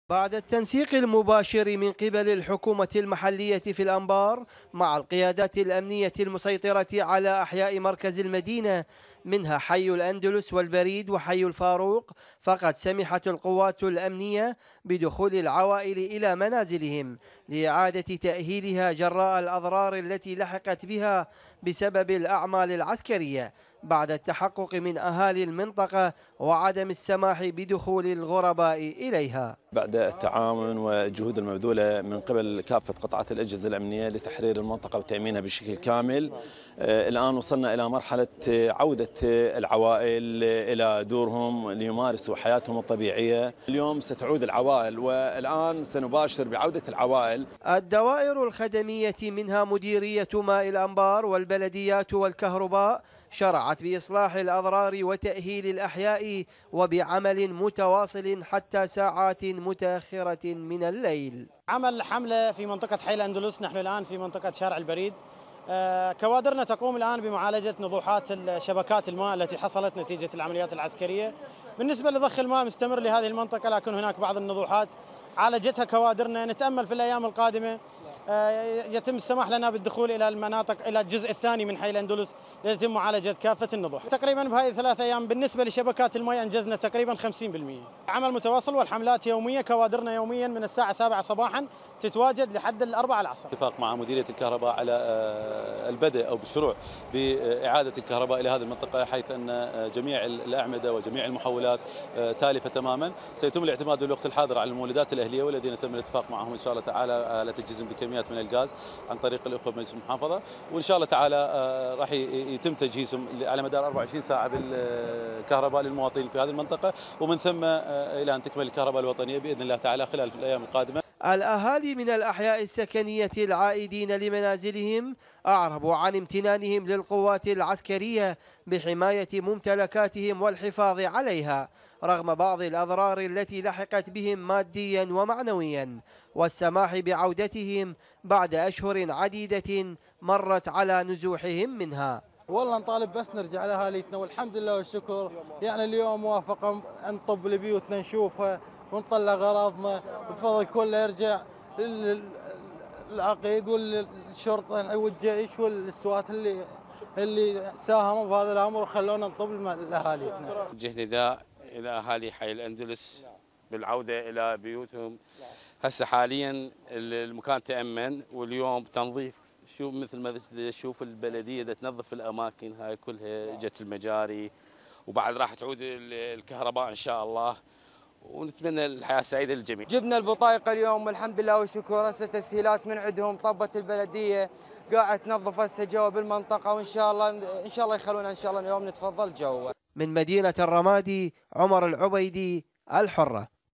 عودة العوائل النازحة الى مناطقهم في حي الأندلس والمعلمين وسط الرمادي وتأهيل الخدمات فيها 07/23
عودة العوائل النازحة الى مناطقهم في حي الأندلس والمعلمين وسط الرمادي وتأهيل الخدمات فيها
عمر العبيدي